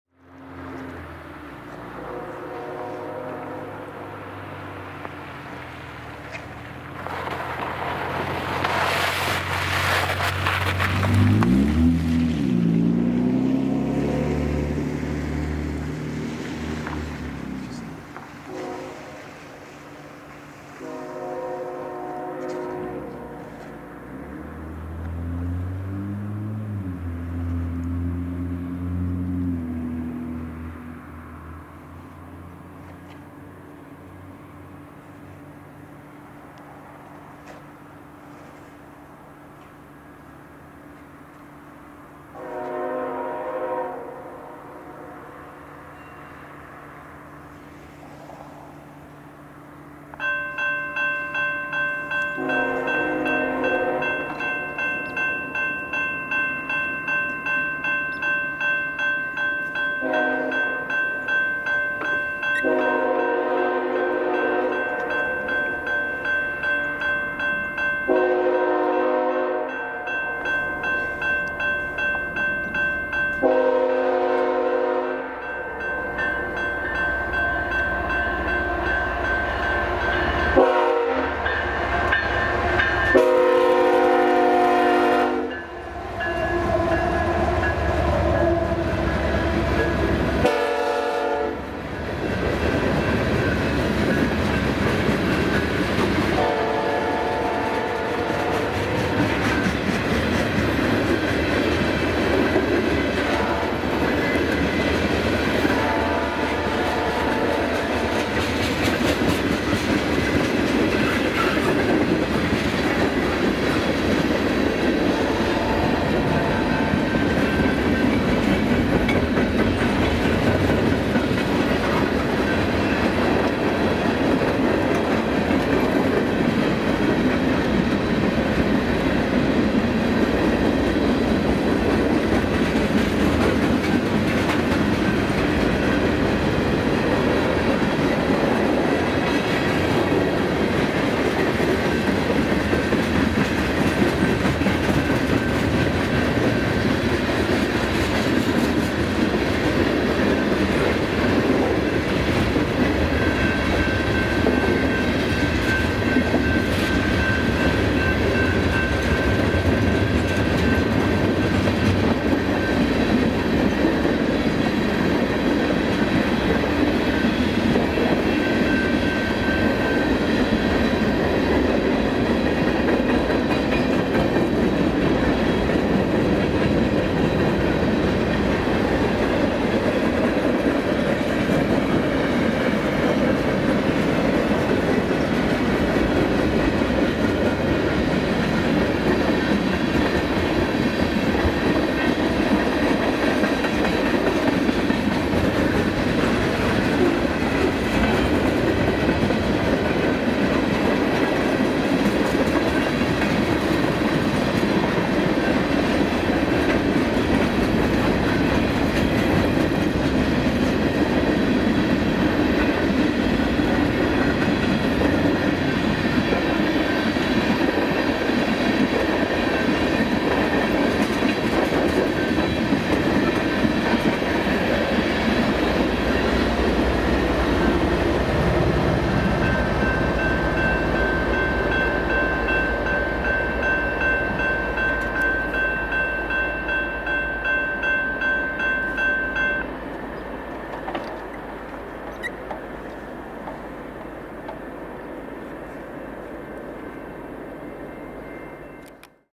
BNSF 3680 West in Dallas City, IL 1/10/24
WB Manifest, Pine St., Dallas City, IL, BNSF Chillicothe Sub
BNSF 3680 = ET44C4 - H3 - RL K5HL
BNSF 5819 = ES44AC - H3
BNSF 7796 = ES44DC - H3 (DPU)